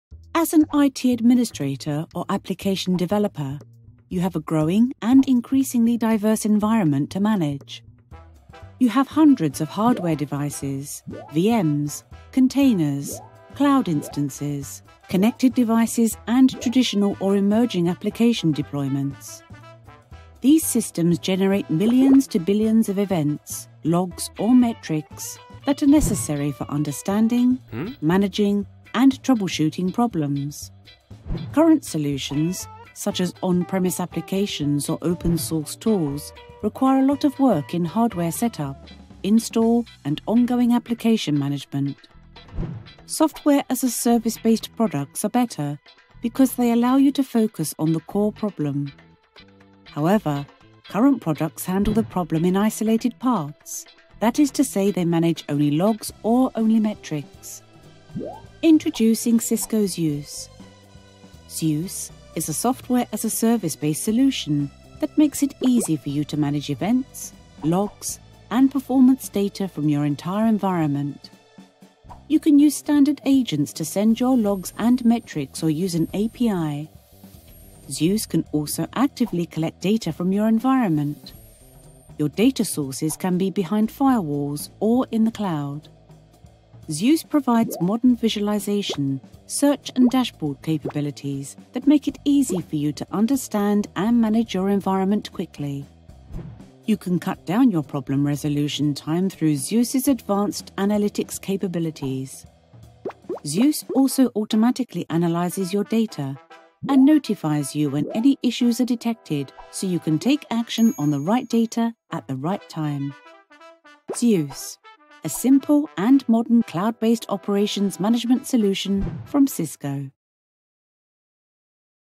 Introducing Cisco Zeus
Introduction video for Zeus, Cisco's Cloud-Based Assurance Service.